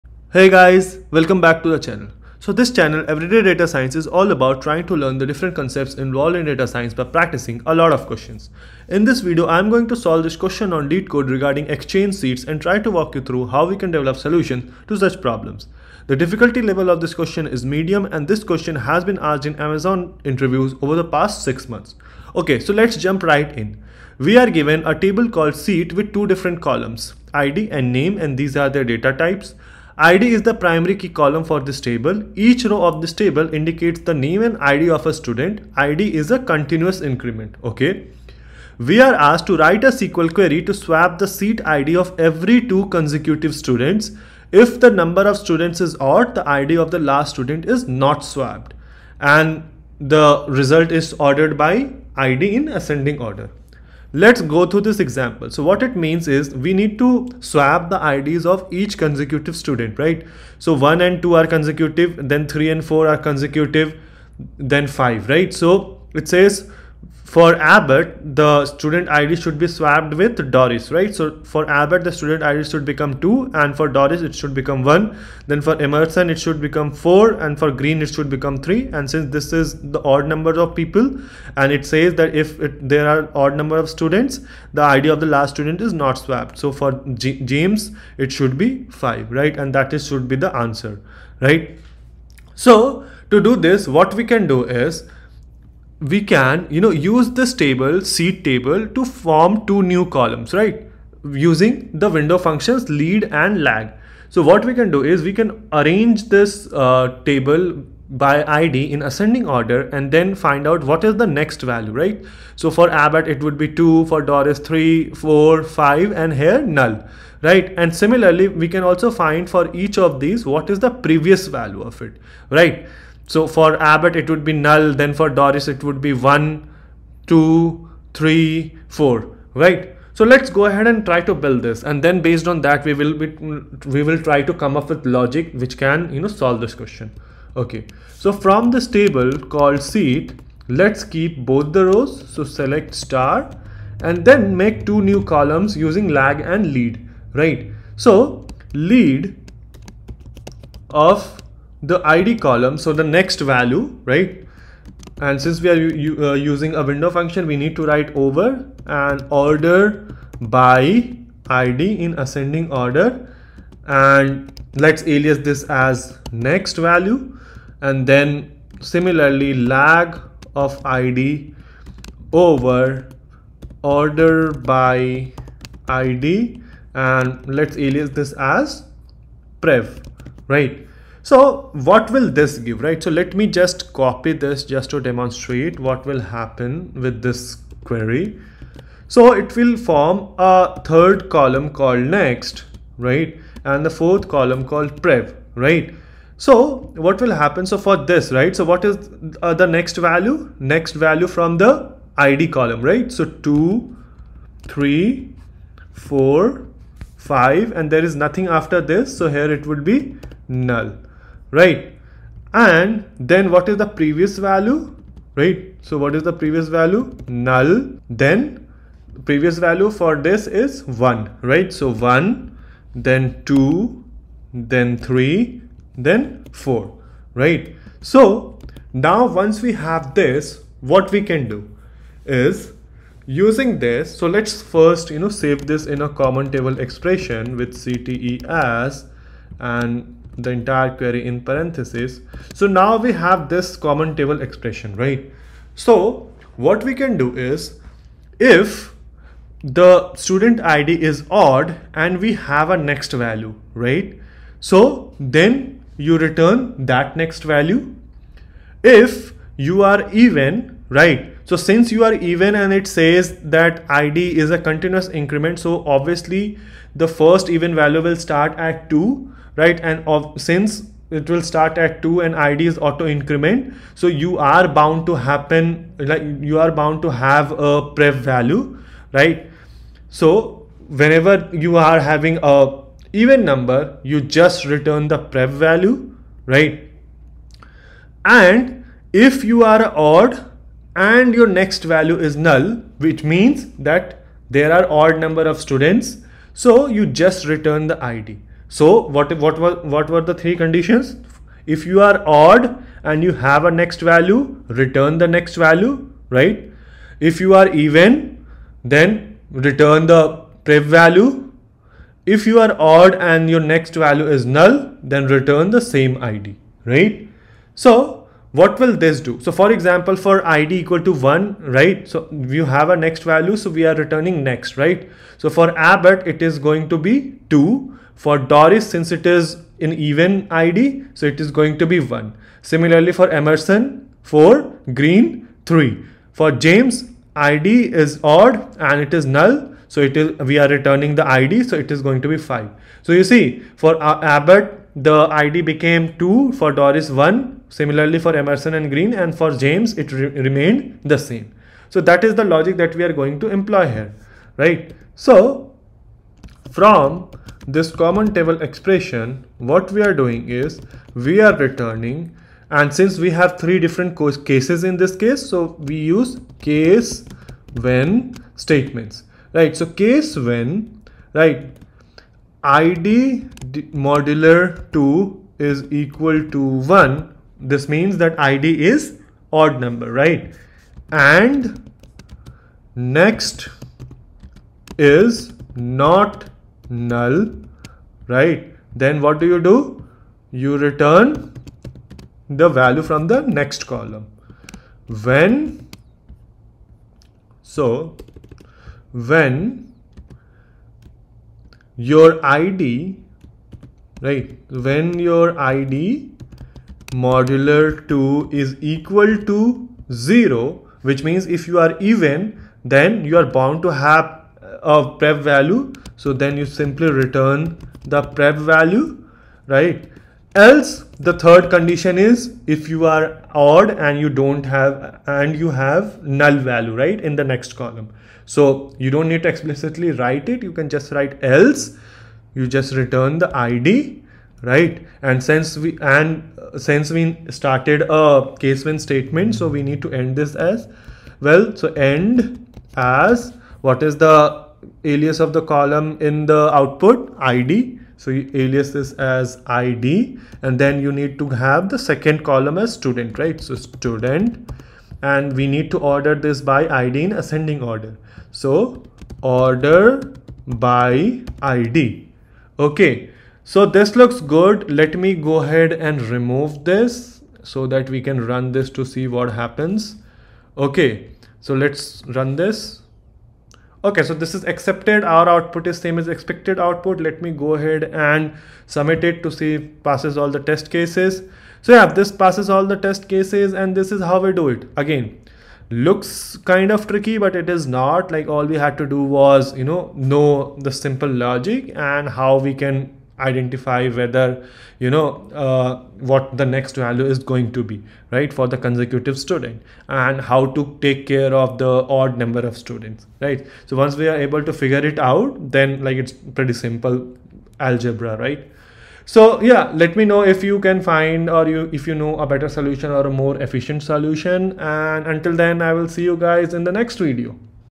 LeetCode Medium 626 Amazon Interview SQL Question with Detailed Explanation
In this video I solve and explain a medium difficulty leetcode SQL question using MySQL query. This question has been asked in Apple, Facebook, Amazon, Google, Adobe, Microsoft, Adobe interviews or what we popularly call FAANG interviews.